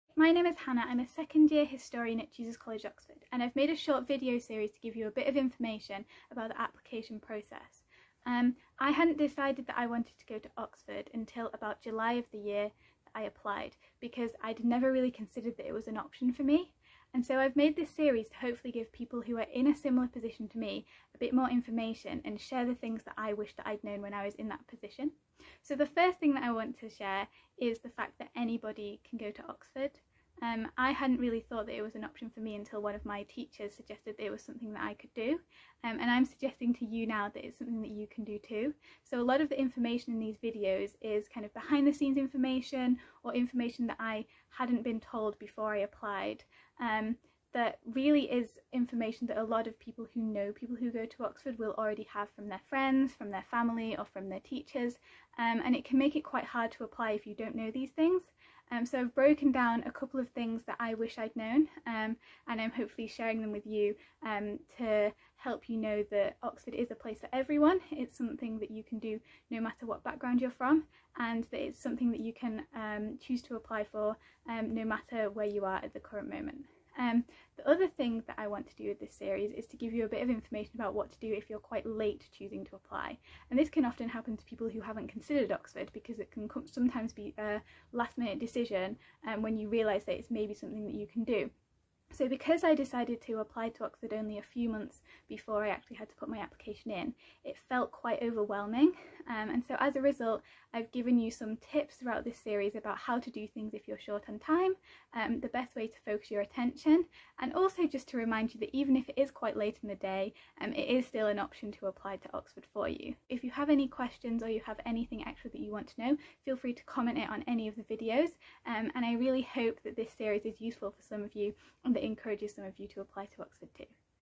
Getting Into Oxford – Introduction
@oxforduniversity undergraduate history student, Hannah Blackmore, gives top tips on getting into the University!